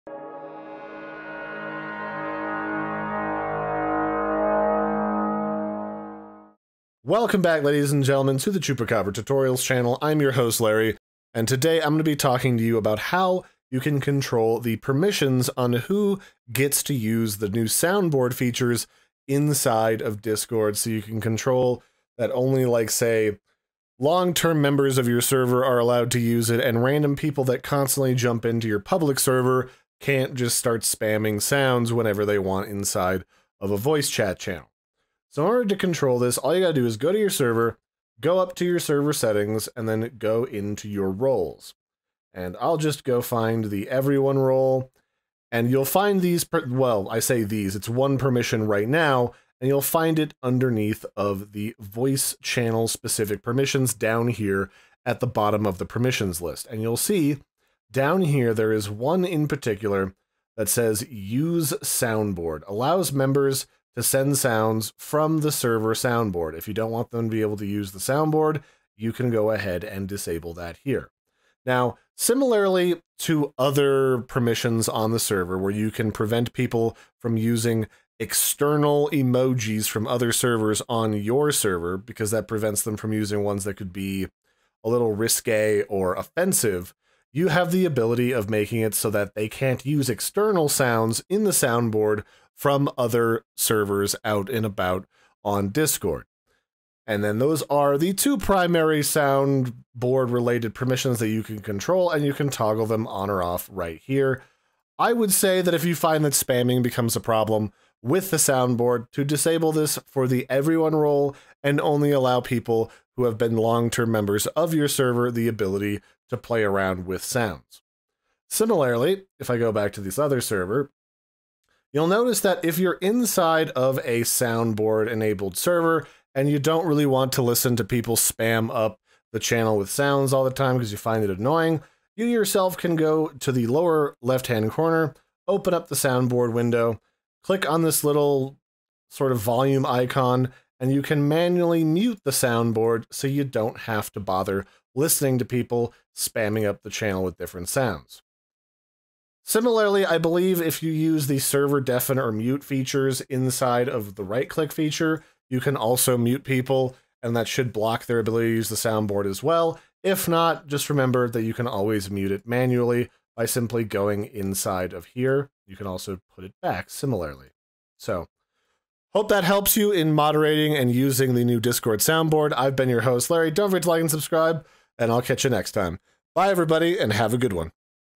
Discord Soundboard Server & Role Permissions Guide - 2023 Tutorial
A quick guide on how to manage the permissions for who can and cant use the new Discord Soundboard on your server. This includes how to block someone from using the new soundboard, and also for how to disallow users from using sounds uploaded to other discord servers.